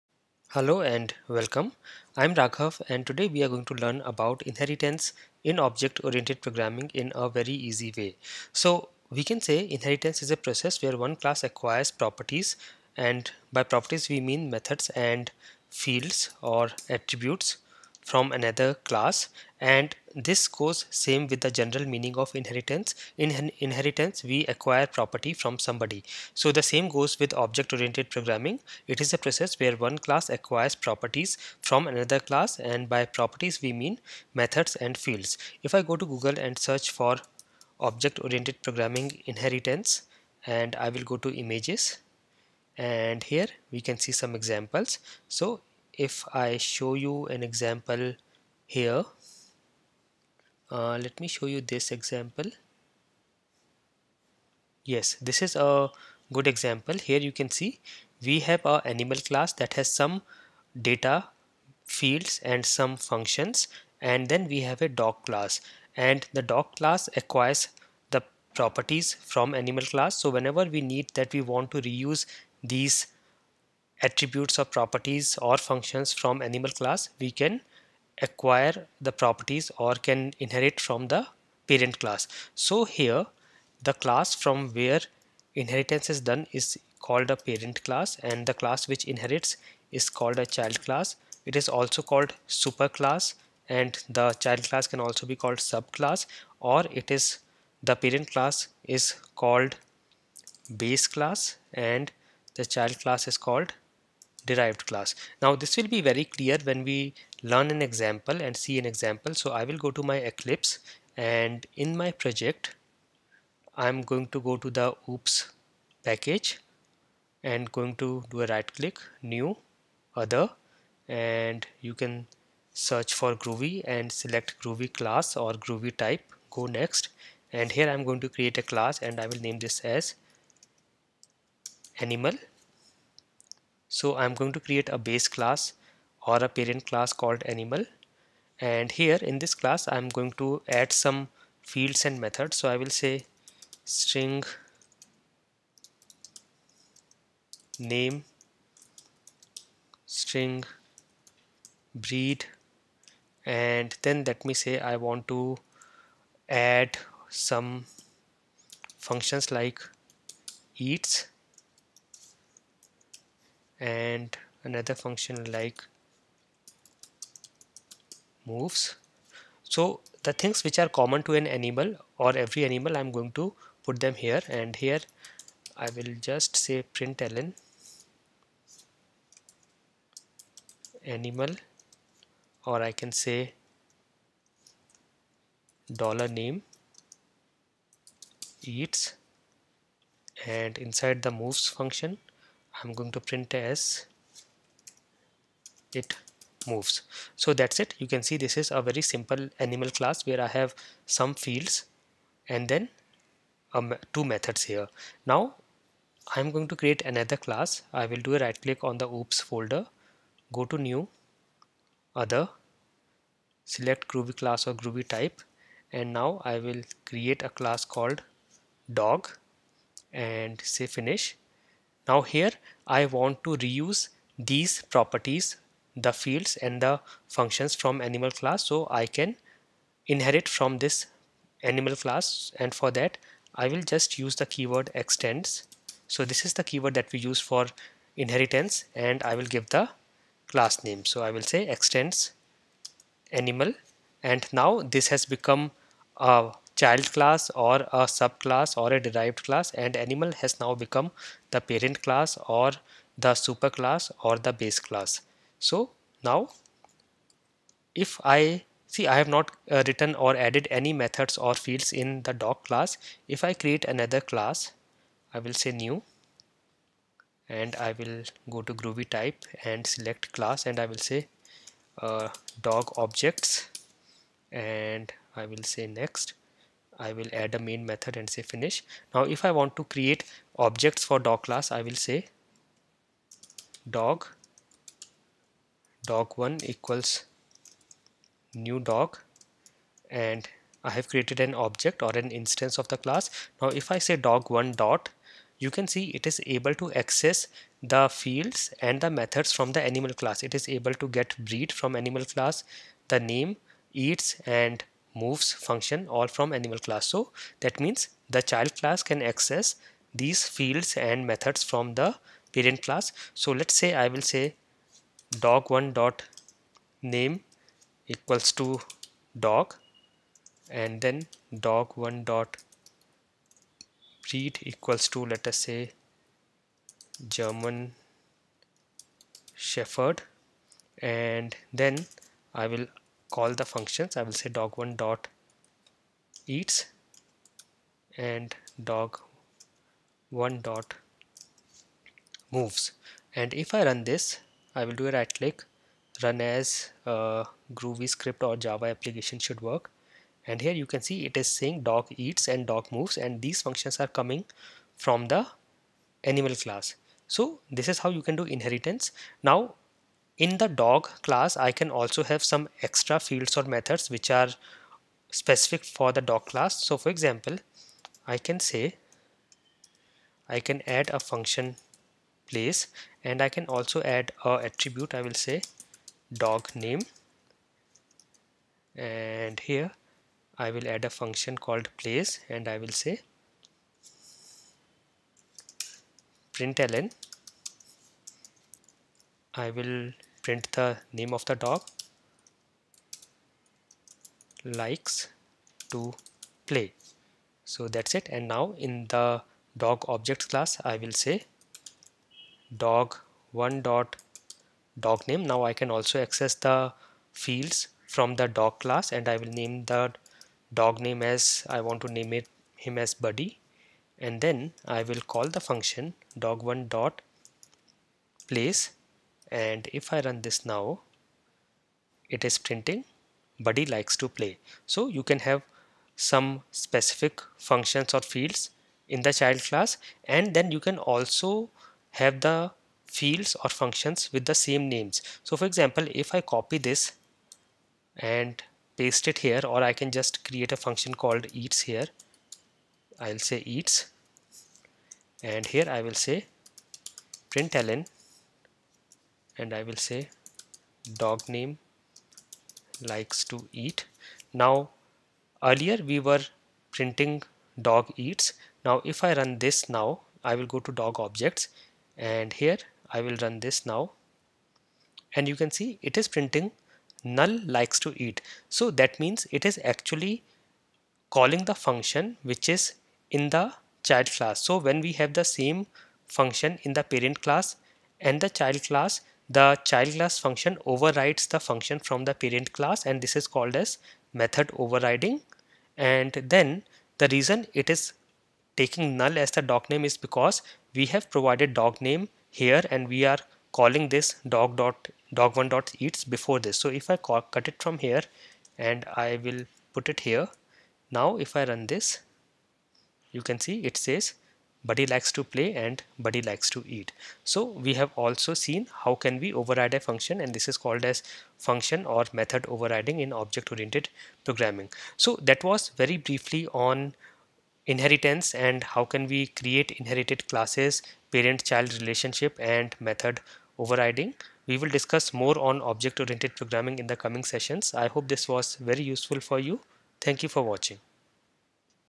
Groovy Beginner Tutorial 27 | OOP | Inheritance and Method Overriding
OOP | Inheritance
Process where one class acquires properties(methods and fields) from another class

parent  child
super sub
base derived

Keep Learning,
Raghav